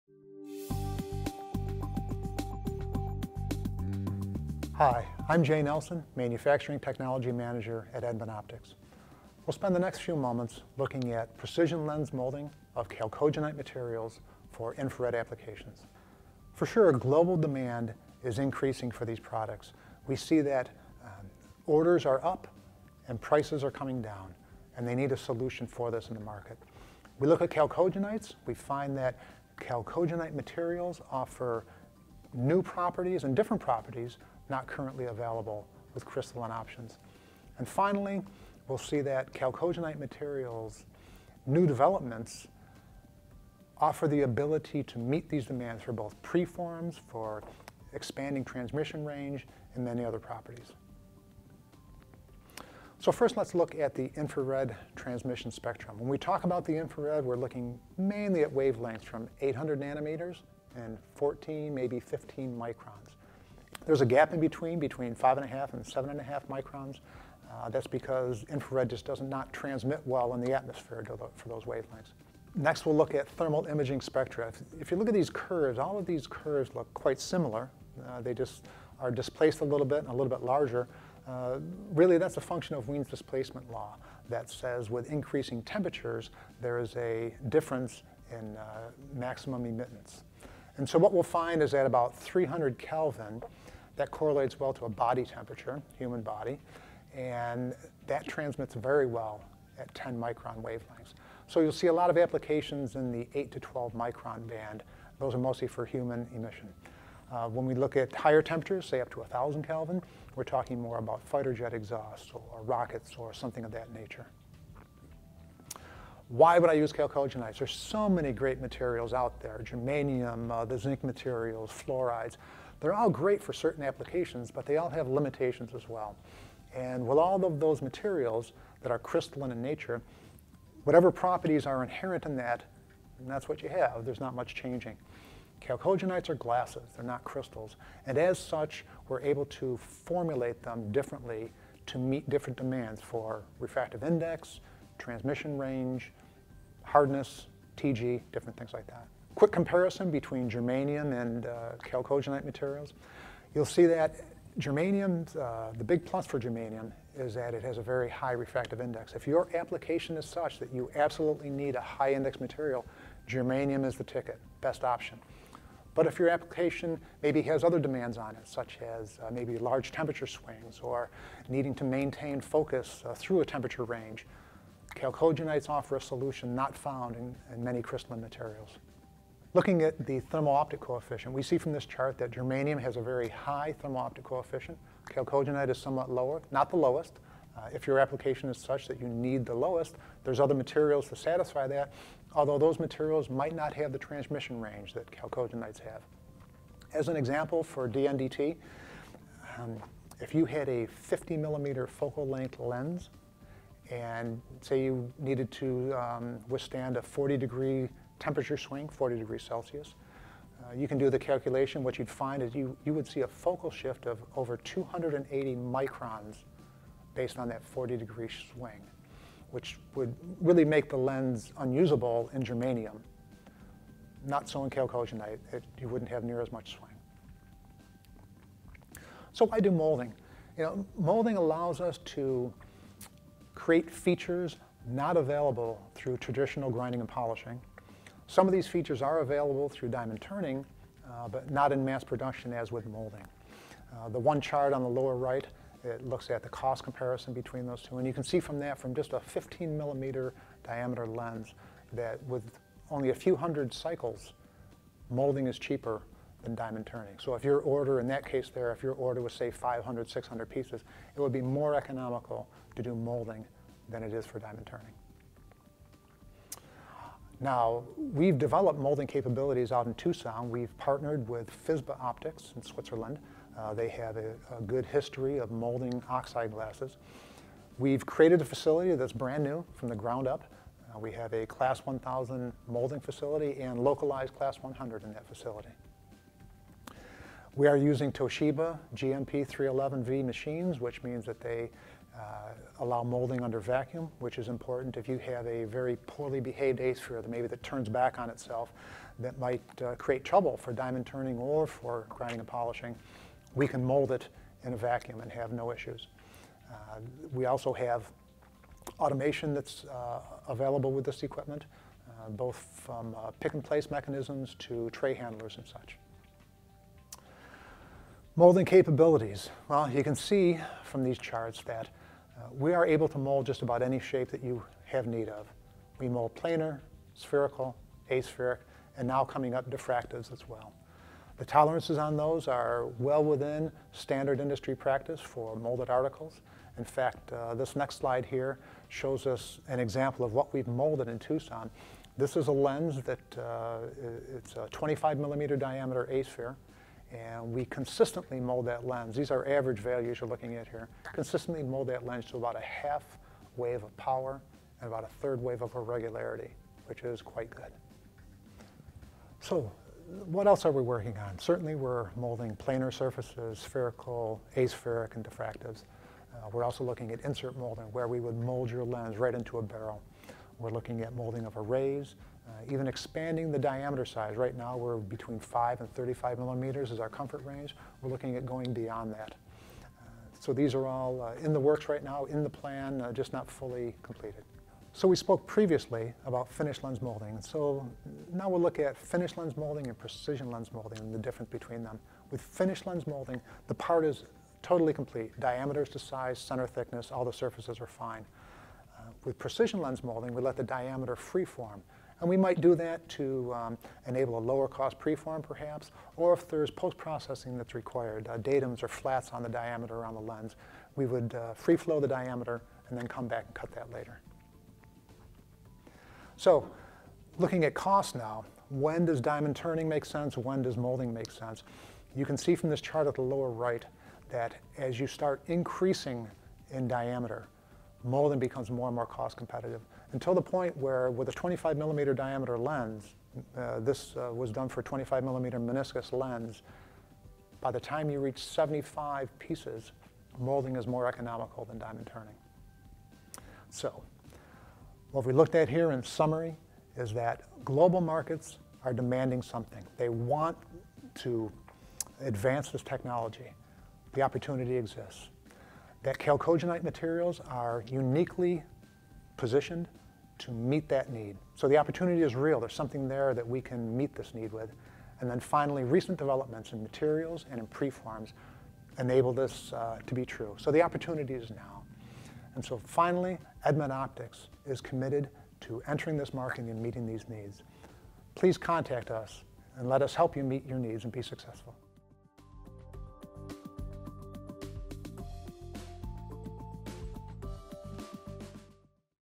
Precision Lens Molding of Chalcogenide Optics
Join Jay Nelson, Manufacturing Technology Manager at Edmund Optics, as he discusses Edmund Optics's chalcogenide molding capabilities, which are ideal for developing infrared optics for a wide range of applications. Some of the topics discussed include the advantages of chalcogenide materials, the benefits of molding, and the capabilities and equipment offered by EO.